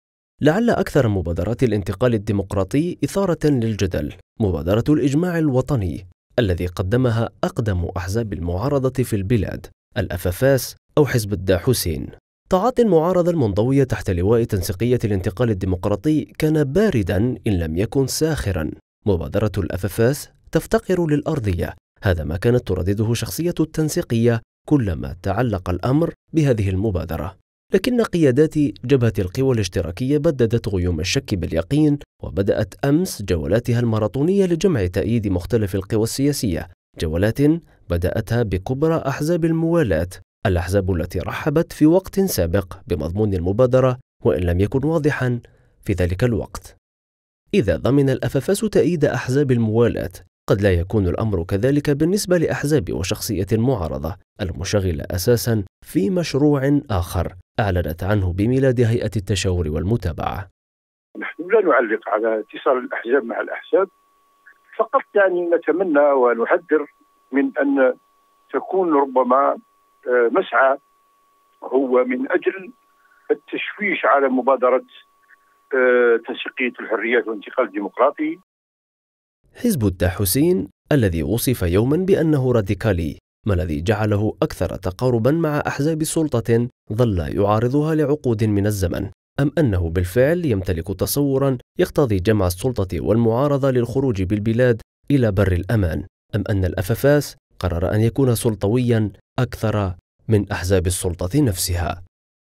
مبادرة الإجماع الوطني .. الأفافاس
جبهة القوى الاشتراكية تشرع في إقناع أحزاب المعارضة والموالاة بمبادرتها ..